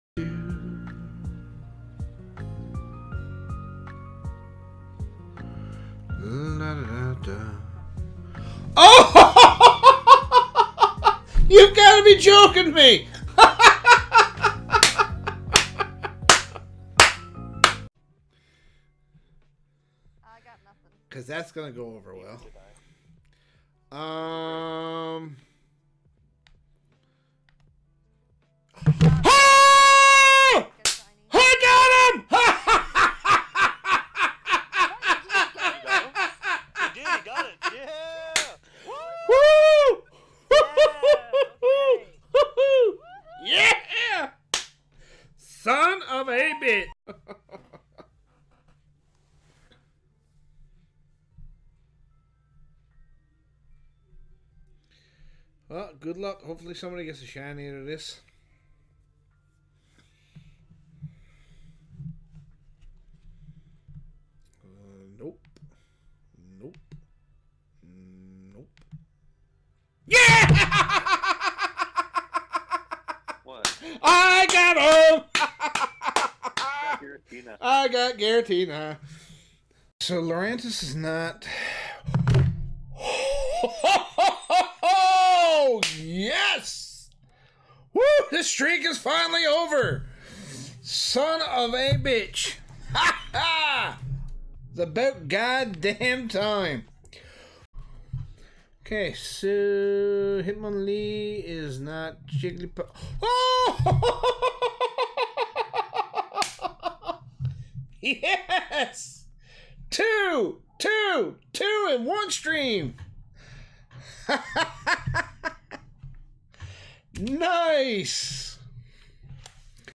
Shiny Collection of surprises and shockers!
Hey folks!! If you enjoy seeing someone react the way I do (or overact. whatever) Then check out this compilation of surprise shinys! And, if you want to see more of this, stop by my channel Come say Hi!!